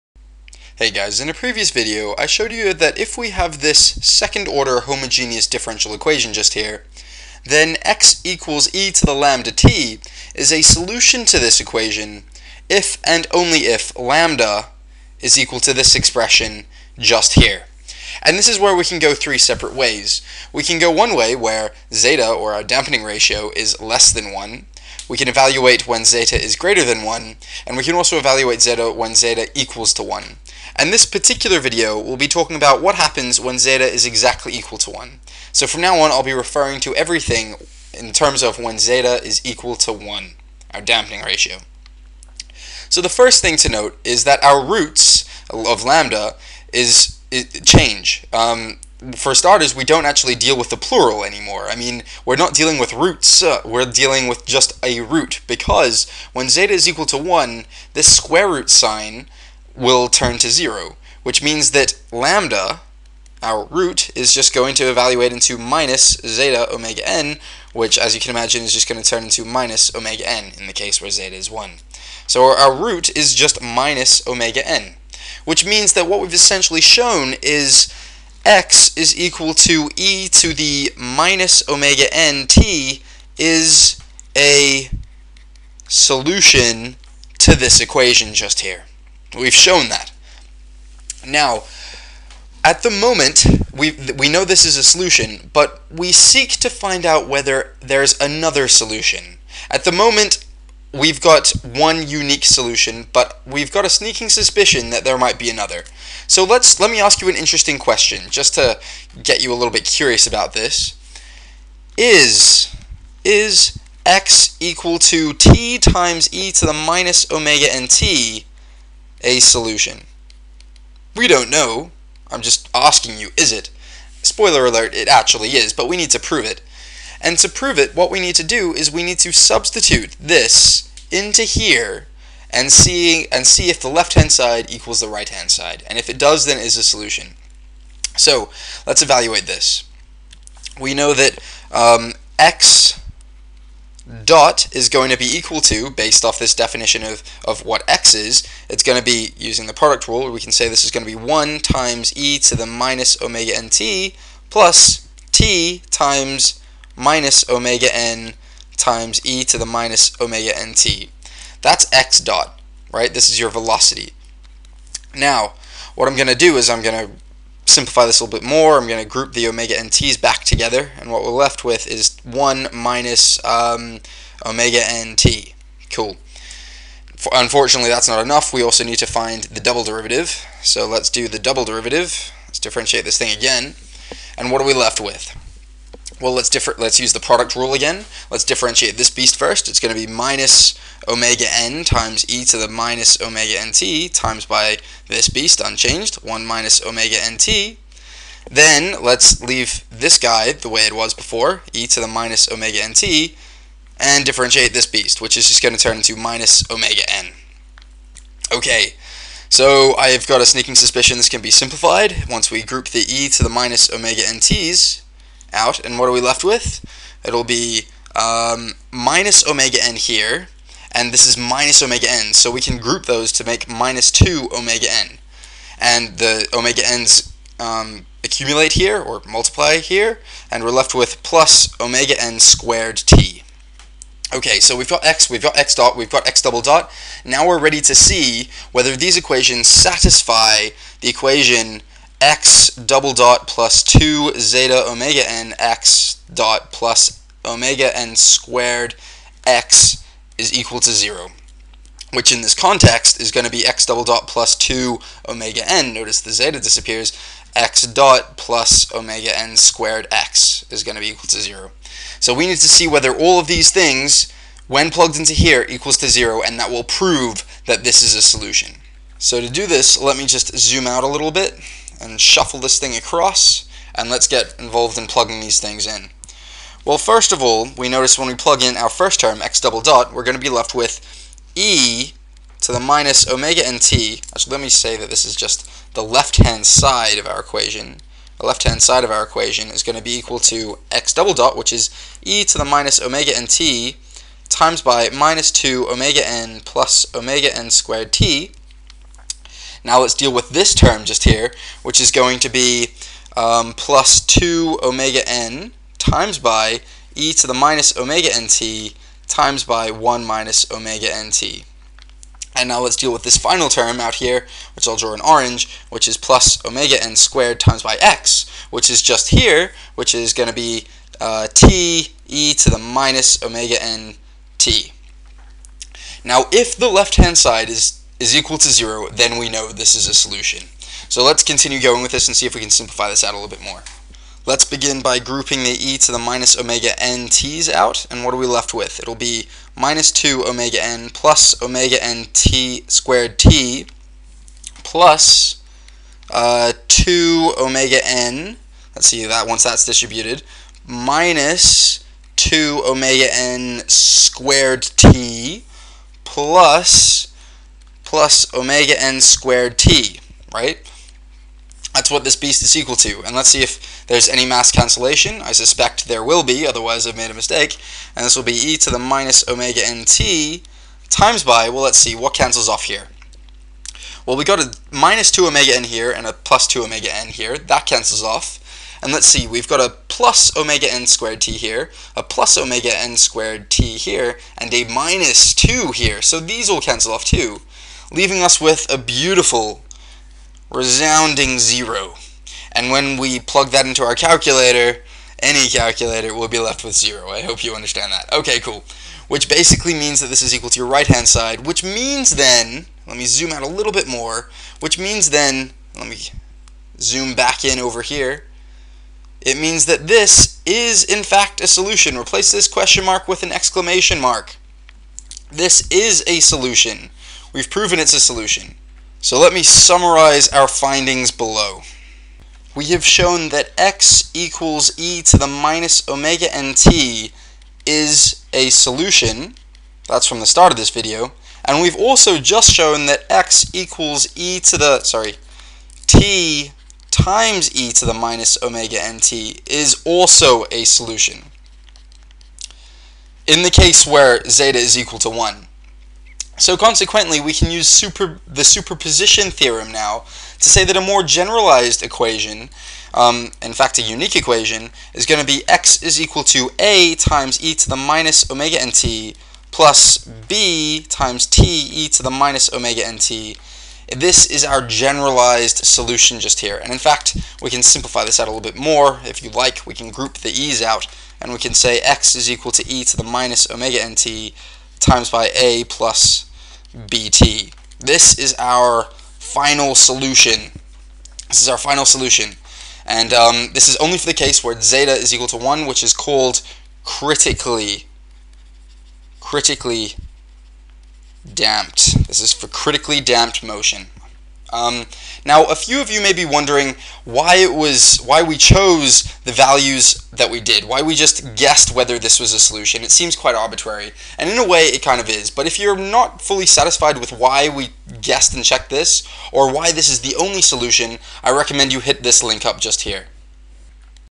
Critically damped motion formula proof (2/2)
A spring mass damper equation when the damping ratio = 1